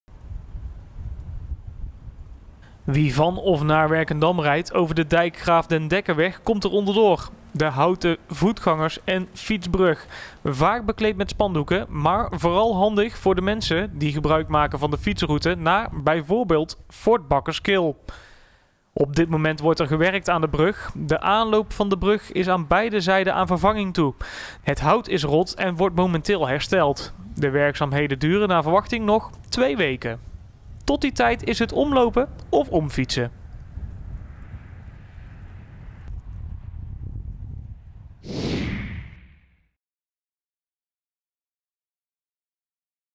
Houten brug over Dijkgraaf den Dekkerweg wordt gerenoveerd - Altena TV
WERKENDAM - De houten loop- en fietsbrug over de Dijkgraaf den Dekkerweg in Werkendam wordt gerenoveerd.

De aanbruggen zijn al enige tijd aan vervanging toe. Het hout op de aanbruggen is in zeer slechte staat. Daarom is men deze week begonnen aan de herstelwerkzaamheden. De brug wordt vaak gebruikt voor fietsverkeer naar bijvoorbeeld Fort Bakkerskil. De brug verbindt de Schans met de Krouwerskade. Het bovenste deel van de brug, de hoofdoverspanning, is minder oud en dus niet aan vervanging toe. Dit werd al eerder vervangen door ongelukken met te hoog verkeer in 2012 en 2014.

Naar verwachting gaan de werkzaamheden 2 weken duren, tot die tijd moet men omlopen of omfietsen.